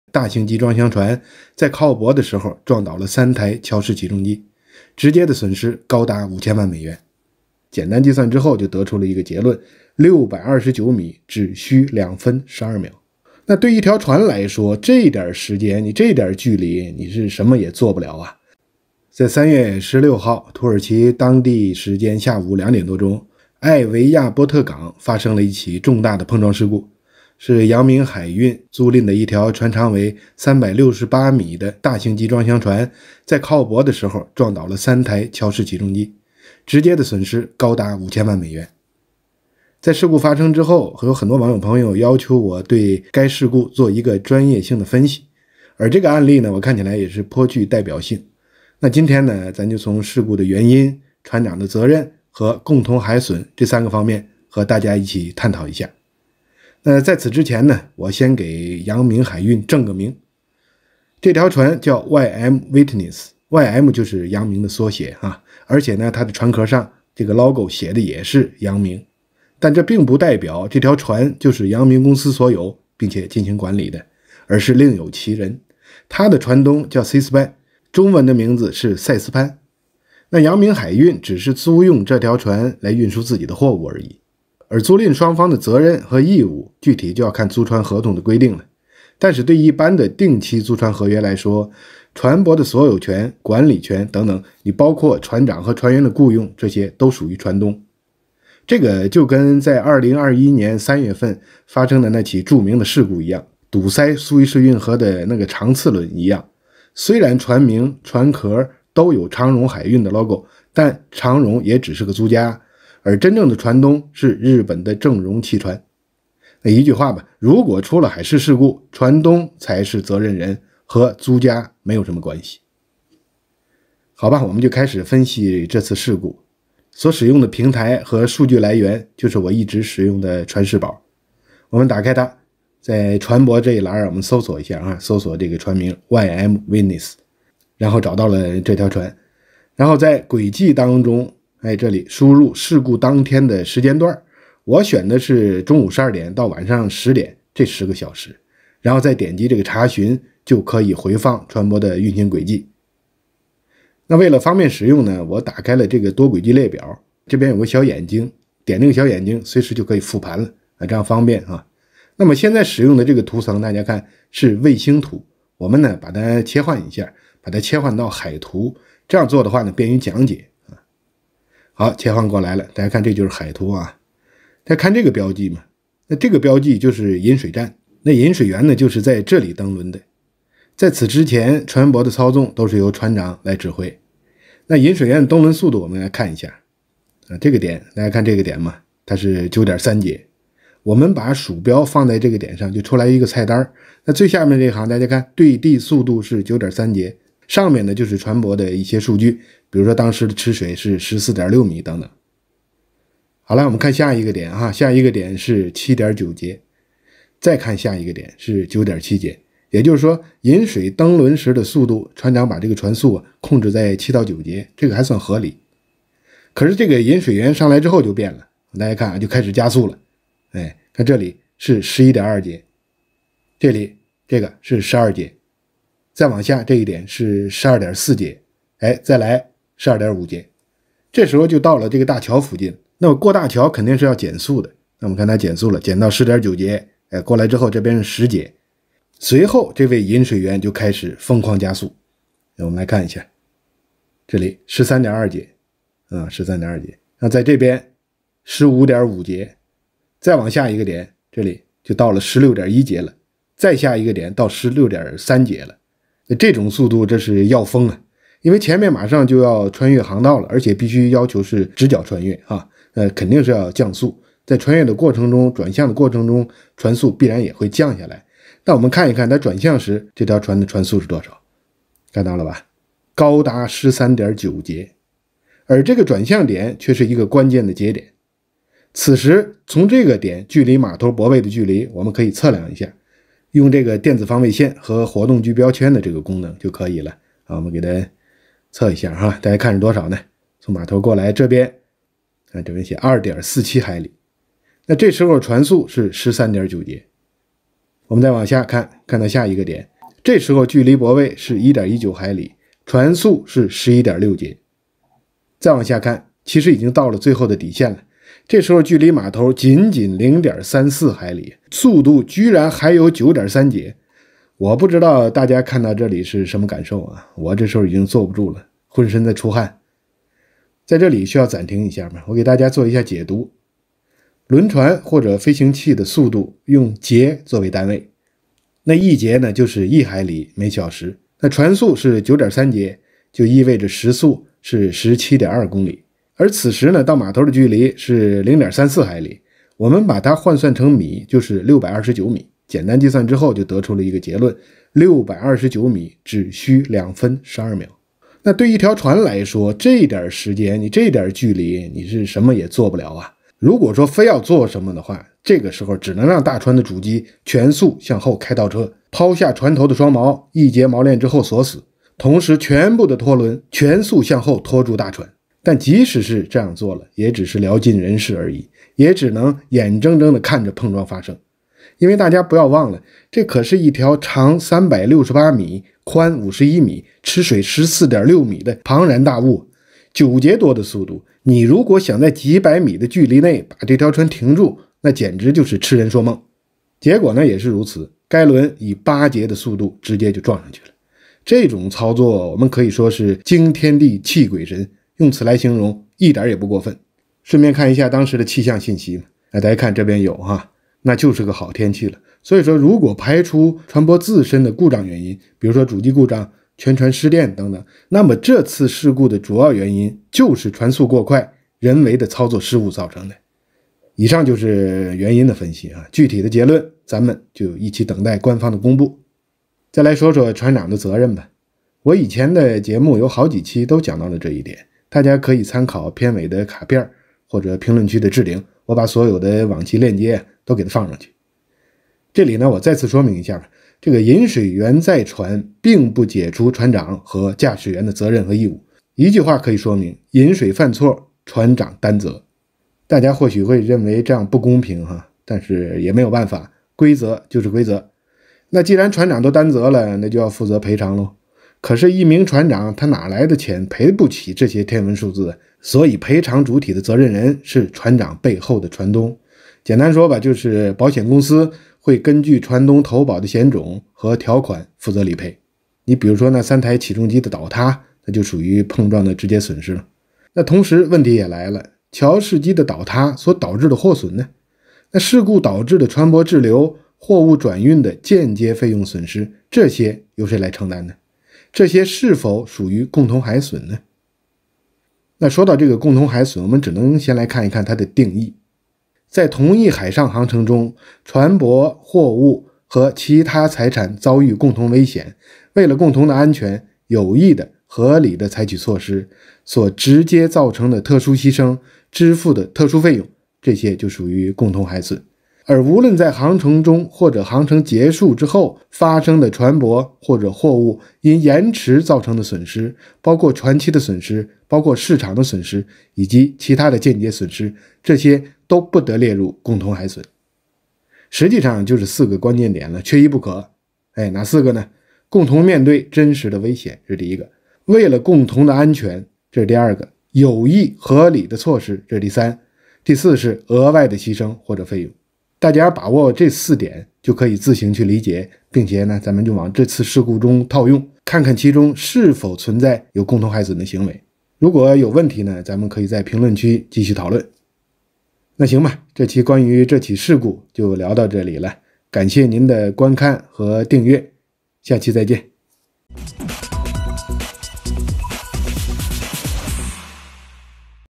損失近16億！貨輪撞倒土耳其港口3起重機 他揭背後真相：引航員瘋了－全民話燒
影音中心／陳瑞婷報導

由我國海運公司「陽明海運」所承租的「視明輪」（YM Witness），在16日停靠土耳其的港口時發生意外，巨大貨輪撞擊港邊大型貨櫃起重機，3架起重機如骨牌般一架接著一架倒塌，損失恐高達5000萬美元（約新台幣16億元），對此，專門跑遠洋商船的李船長，日前利用「船視寶」平台來分析整起事件的來龍去脈，不禁直呼「引航員瘋了」。